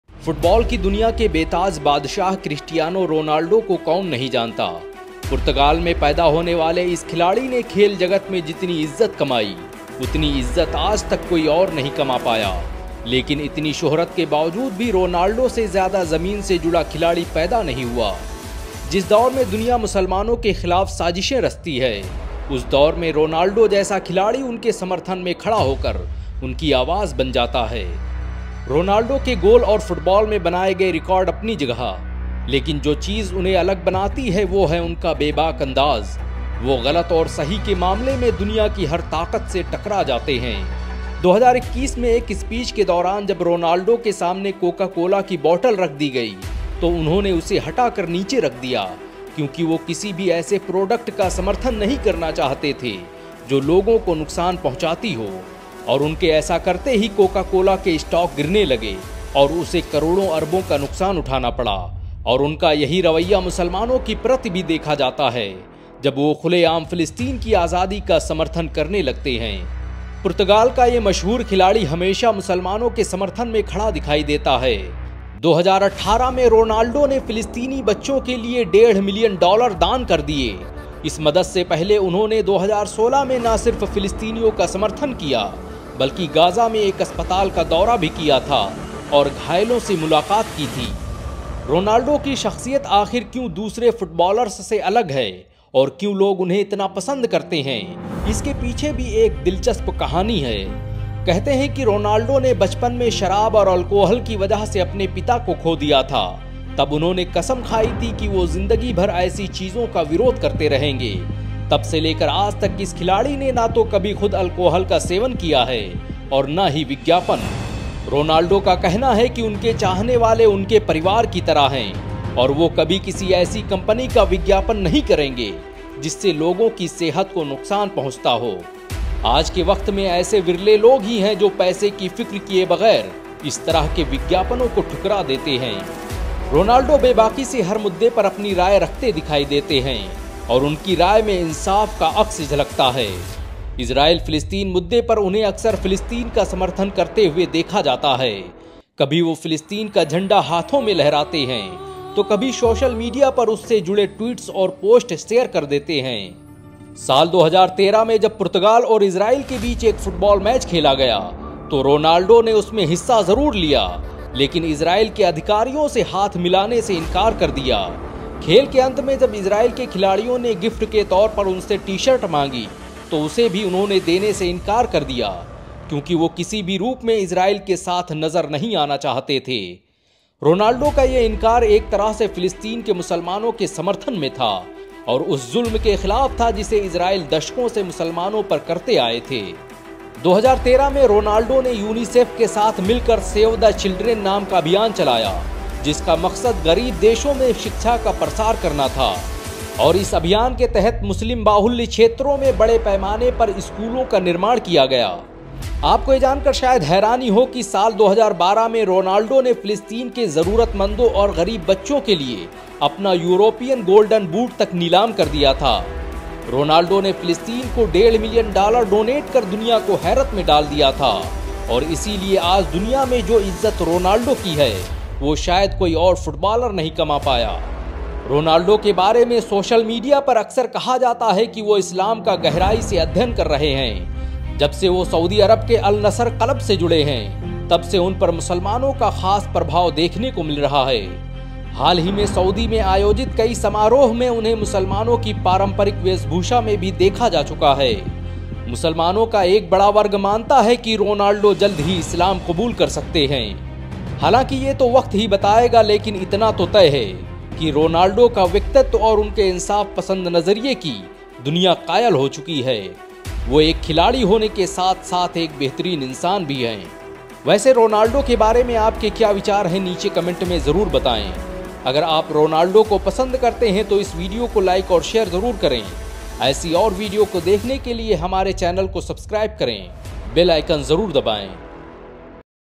क्या रोनाल्डो इस्लाम अपनाने वाले हैं ? रोनाल्डो इजरायल से नफरत क्यूं करते हैं? | Ronaldo and Islam
In this video we’re talking about Cristiano Ronaldo and his love for Muslims and Islam. famous Portuguese player Ronaldo is always seen standing in support of Muslims. In 2018, Ronaldo donated $1.5 million for Palestinian children. Before this help, in 2016, he not only supported the Palestinians but also visited a hospital in Gaza and met the injured. Now he studying Islam deeply and some rumers say that he may be accepted Islam soon.

इस वीडियो में पुर्तगाल के मशहूर खिलाड़ी क्रिस्टियानो रोनाल्डो के बारे में बात की गई है. पुर्तगाल में पैदा होने वाले इस खिलाड़ी ने खेल जगत में जितनी इज्जत कमाई उतनी इज्जत आज तक कोई और नहीं कमा पाया..लेकिन इतनी शोहरत के बावजूद भी रोनाल्डो से ज़्यादा जमीन से जुड़ा खिलाड़ी नहीं पैदा हुआ. जिस दौर में दुनिया मुसलमानों के खिलाफ साजिशें रचती हैं उस दौर में रोनाल्डो जैसा खिलाड़ी उनके समर्थन में खड़ा होकर उनकी आवाज़ बन जाता है.

रोनाल्डो के गोल और फुटबाल में बनाए गए रिकार्ड अपनी जगह लेकिन जो चीज उन्हें अलग बनाती है वोह है उनका बेबाक अंदाज... वोह गलत और सही के मामले में दुनिया की हर ताकत से टकरा जाते हैं. 2021 में एक स्पीच के दौरान जब रोनाल्डो के सामने कोका कोला की बॉटल रख दी गई तो उन्होंने उसे हटा कर नीचे रख दिया क्यूंकि वोह किसी ऐसे प्रोडक्ट का समर्थन नहीं करना चाहते थे जो लोगों को नुक्सान पहुंचाती हो और उनके ऐसा करते ही कोकाकोला के स्टॉक गिरने लगे और उसे करोड़ों अरबों का नुक्सान उठाना पड़ा और उनका यही रवैया मुसलमानों के प्रति भी देखा जाता है जब वोह खुलेआम फिलिस्तीन की आज़ादी का समर्थन करने लगते हैं.